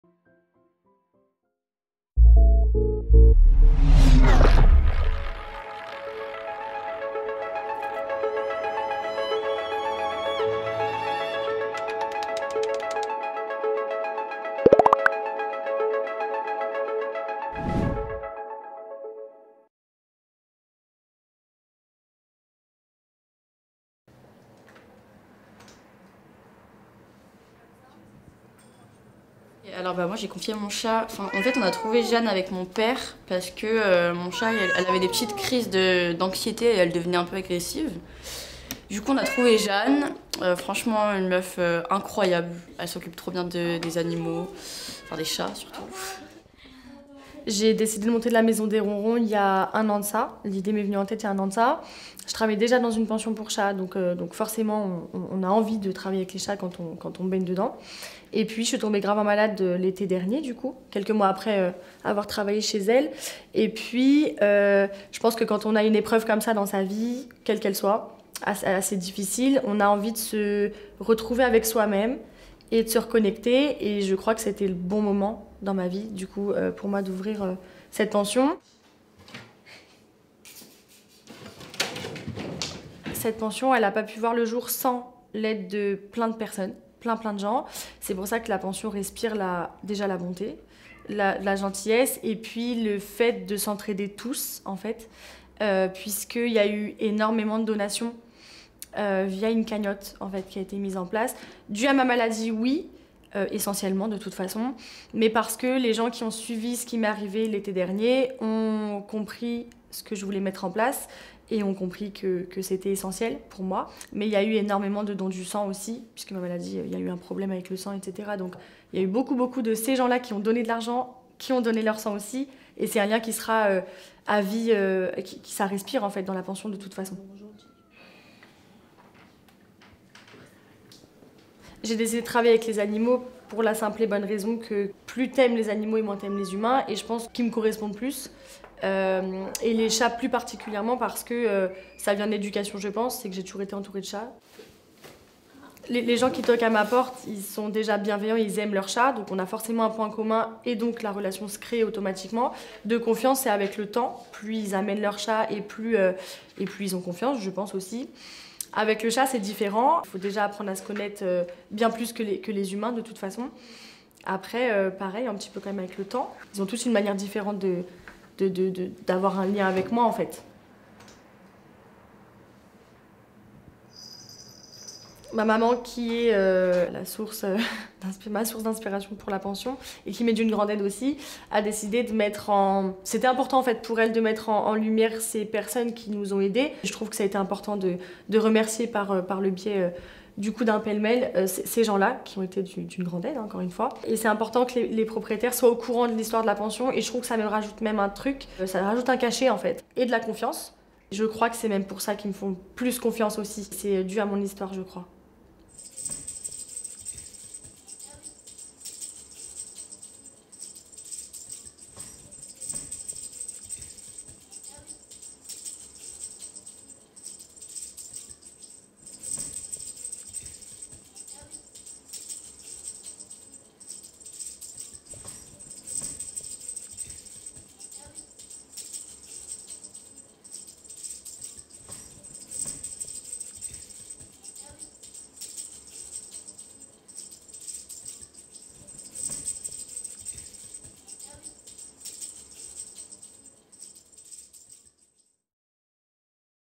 Reportage étudiant - La maison des ronrons
Découvrez le reportage étudiant "La maison des ronrons", réalisé par les étudiants de l'ÉSEC Lyon.